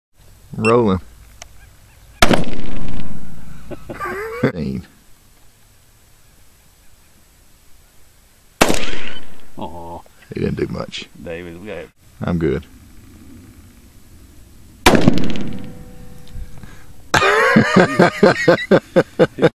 Prairie dog shots
Shooting Prairie dogs in WY